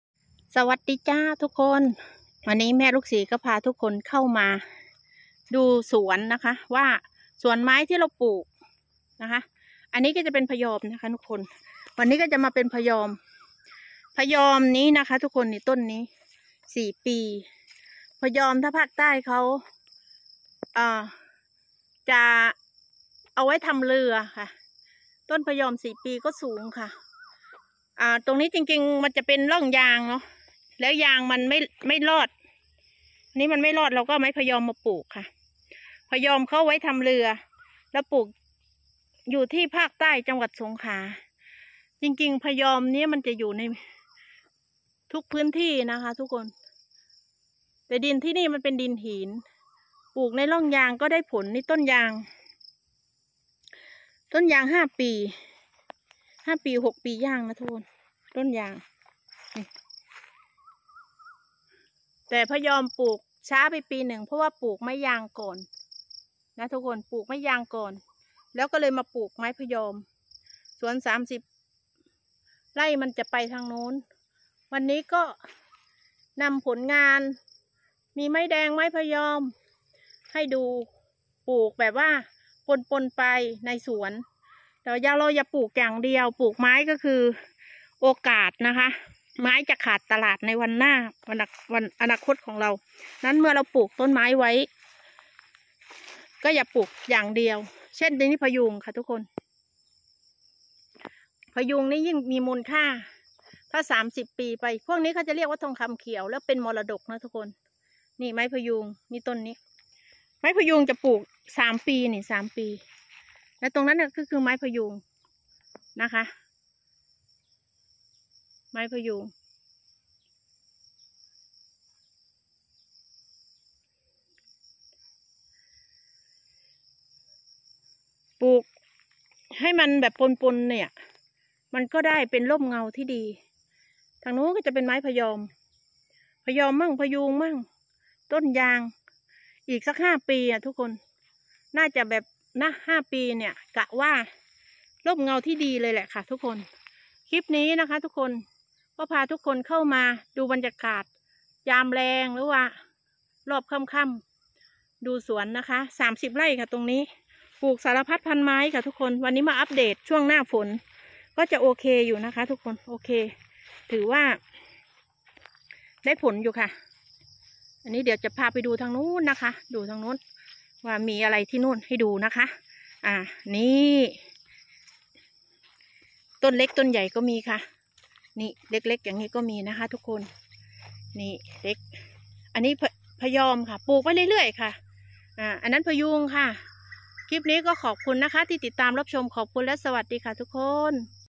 สวนยางสวนเกษตรผสมผสาน
บรรยากาศในสวนร่มรื่นมีต้นไม้เยอะ